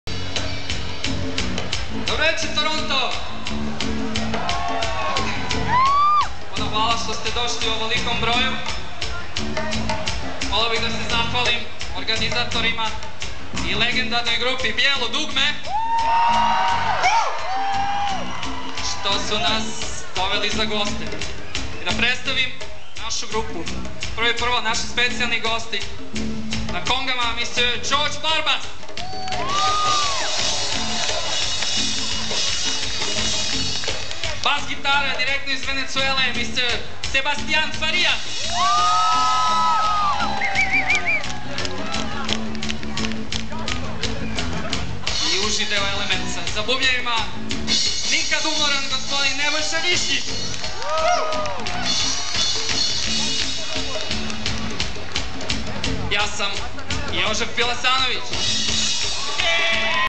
ElementZ gig at the Sound Academy - Introductions
Playing with the band ElementZ at the Sound Academy in Toronto. Opening for Bijelo Dugme.